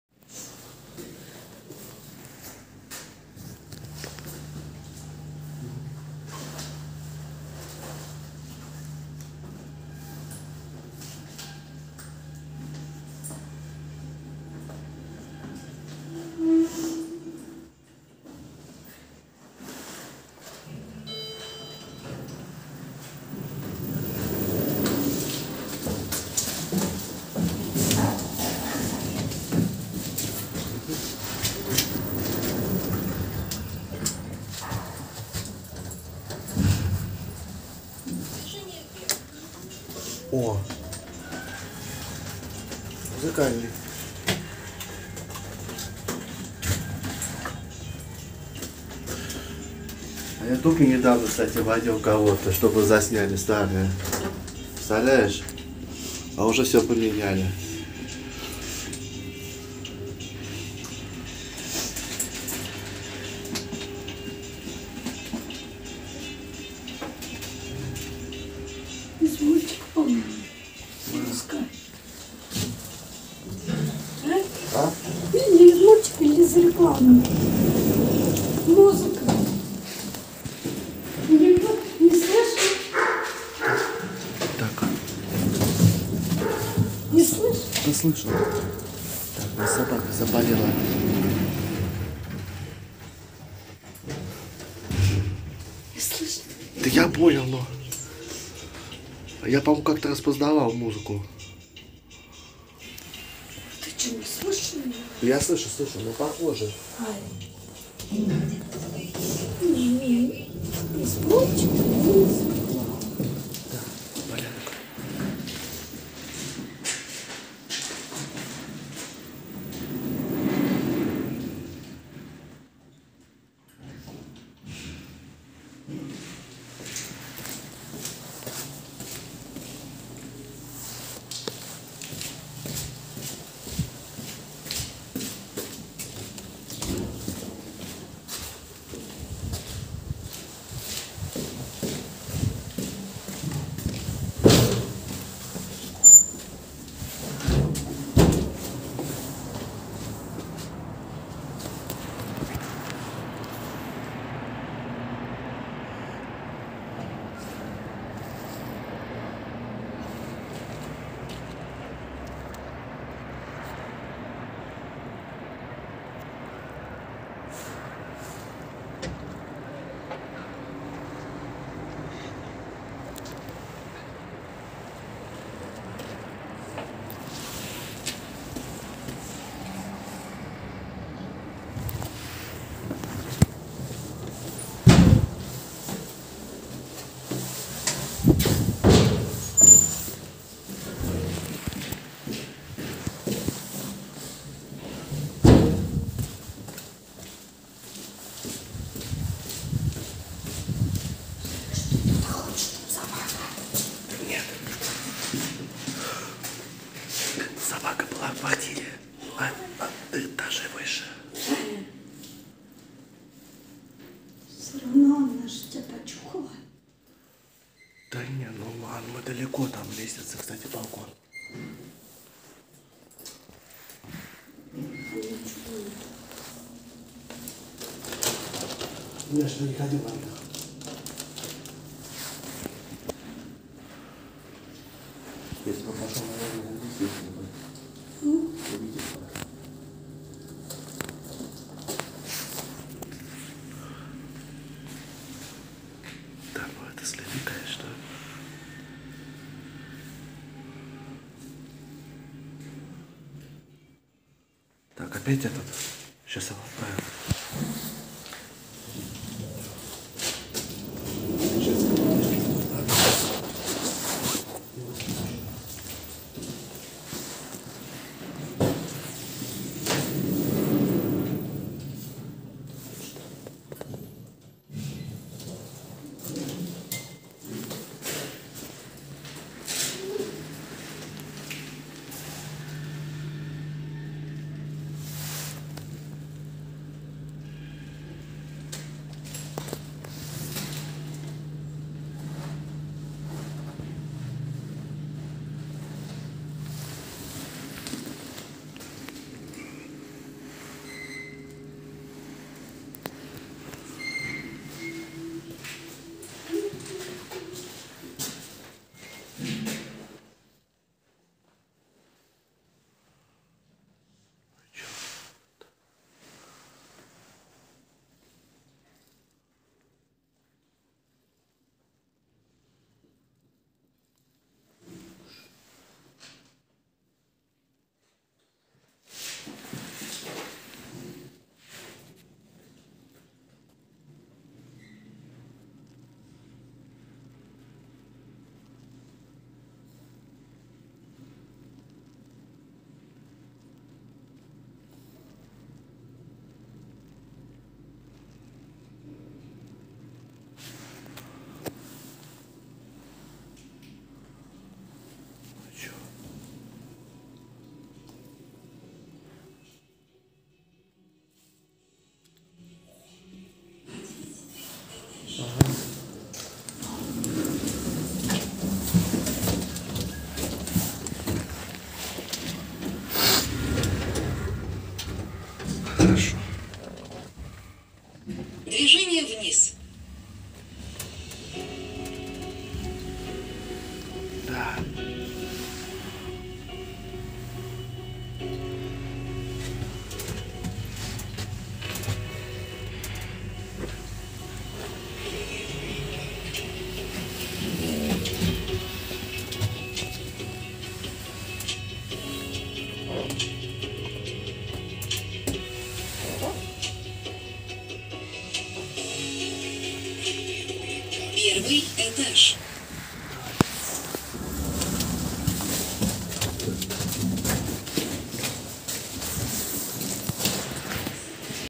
Грузопассажирский, пассажирский лифт (деревня Малые Вязёмы), V=1 м-с, г-п 400, 630 кг (3)
Петровское шоссе 1
Ссылка на лифты до замены в этом доме: